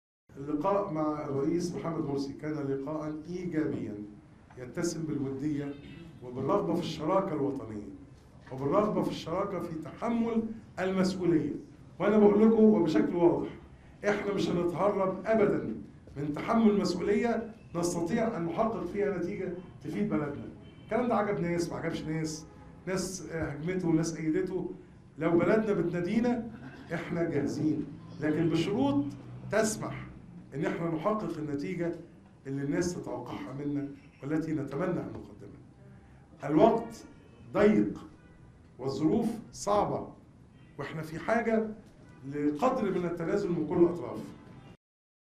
نور: الحل في حكومة إنقاذ وطني
قال زعيم حزب غد الثورة المصري أيمن نور إن اللقاء الذي جمعه بالرئيس المصري محمد مرسي كان إيجابيا، وأوضح أنه اتفق مع عمرو موسى على أن حل الأزمة يمر عبر تأجيل الانتخابات، وتشكيل حكومة إنقاذ وطني.